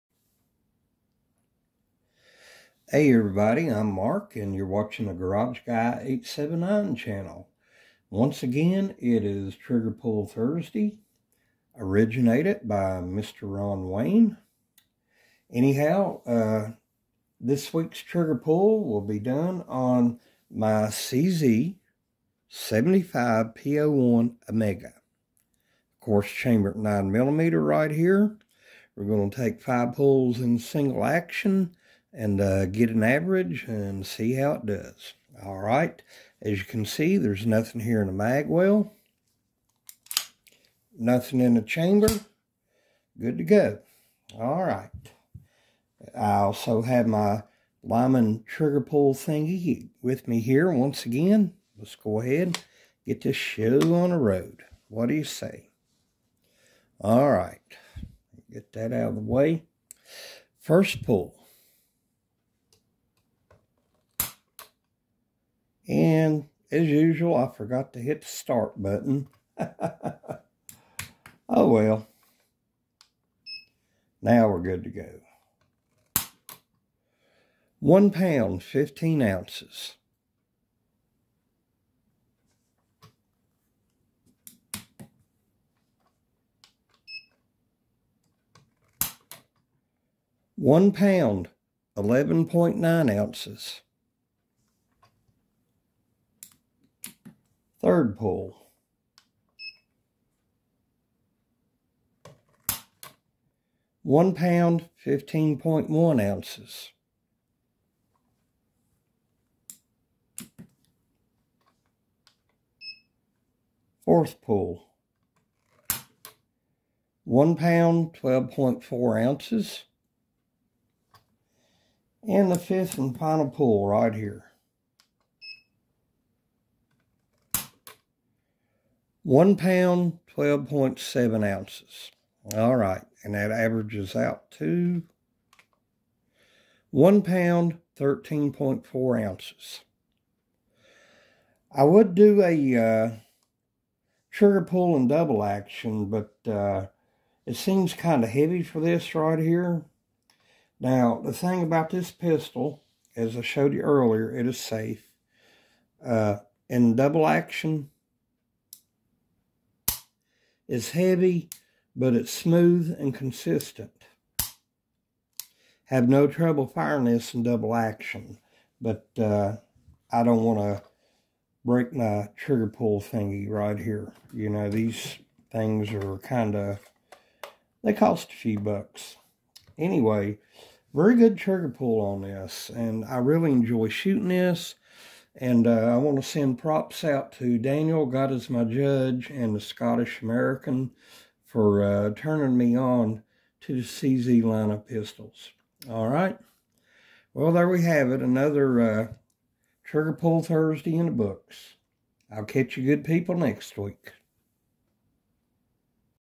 triggerpullthursday CZ 75 P-01 Omega
triggerpullthursday cz czechrepublic CZ 75 P-01 Omega 5 pulls in single action.